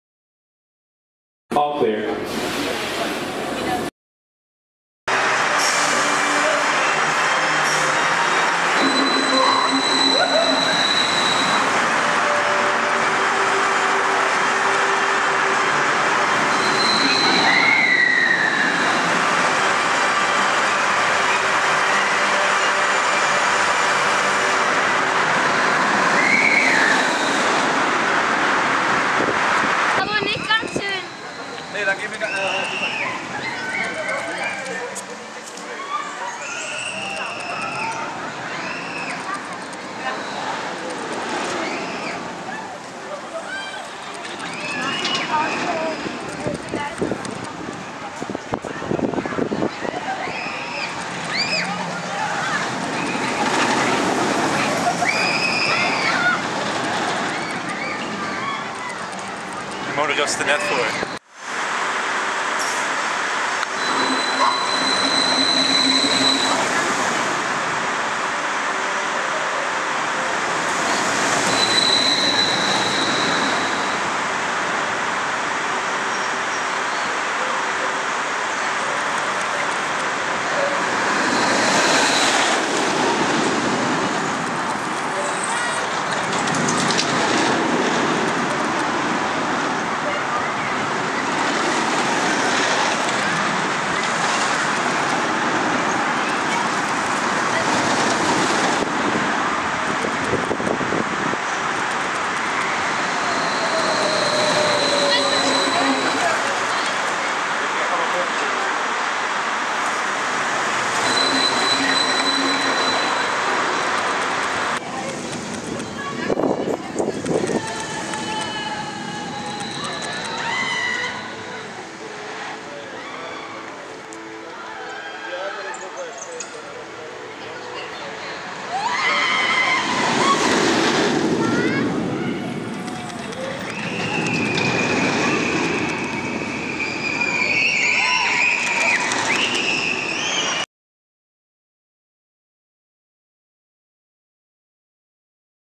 Gold Rush offride - Slagharen - 4k60 copyright free
Gold Rush is a Gerstlauer multilaunch Infinity coaster located in Slagharen.
This is older footage shot with a different camera so the quality will be lower.
This video has been lighty edited so you can choose how to use the clips.

Feel free to use this footage in your commentary or top 10 video.
just mention us in your description.
You may not remove the watermark.
We would love to see your final product! Share your video in the comments!